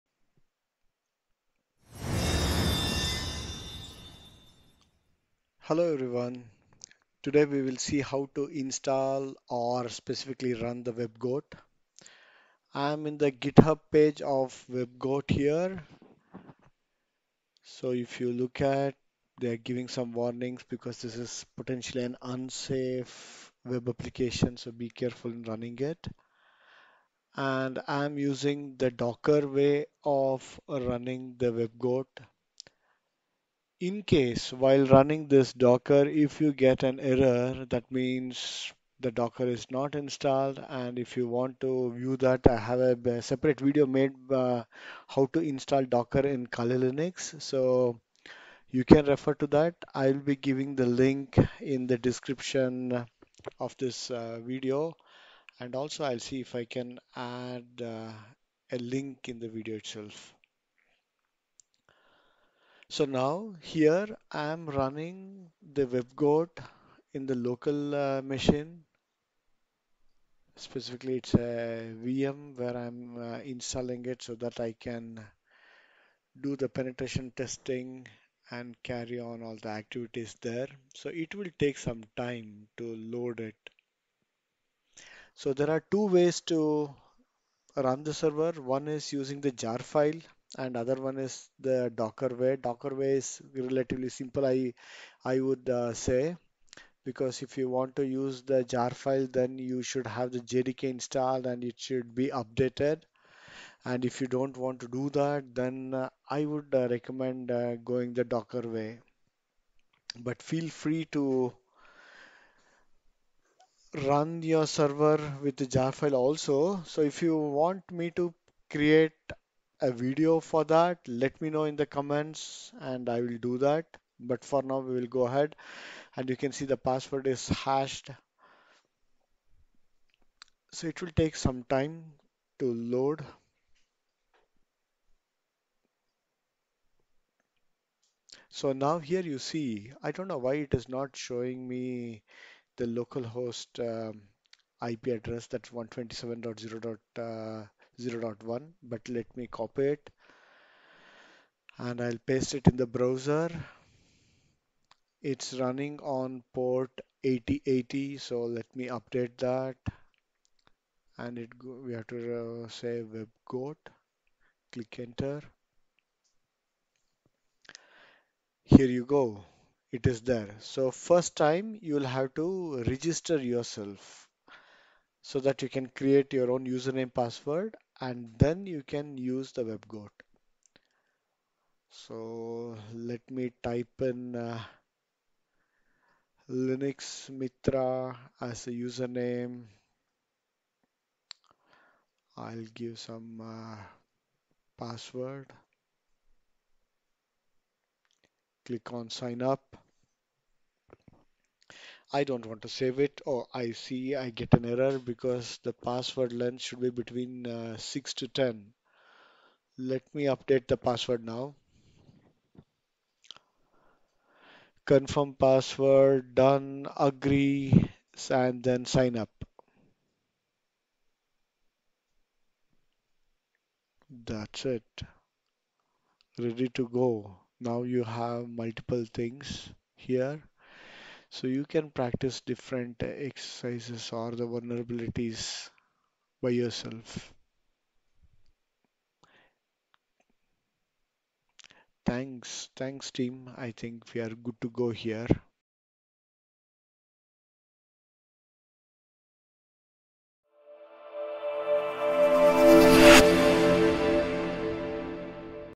Install / Run WebGoat in Kali Linux
This Video demonstrates how to run WebGoat using docker in Kali Linux

If you have not installed Docker in Kali linux then check out the link below on how to install Docker